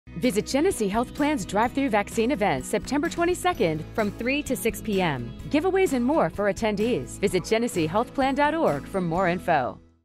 You're Invited! GHP Vaccine Drive-Thru Event Commercial 9.22.23
You're invited to GHP's Vaccine Drive-Thru Event on Friday, 9/22/23 from 3-6p.m. Flu vaccines will be available, along with FREE ice cream, giveaways, health care enrollment resources, and more!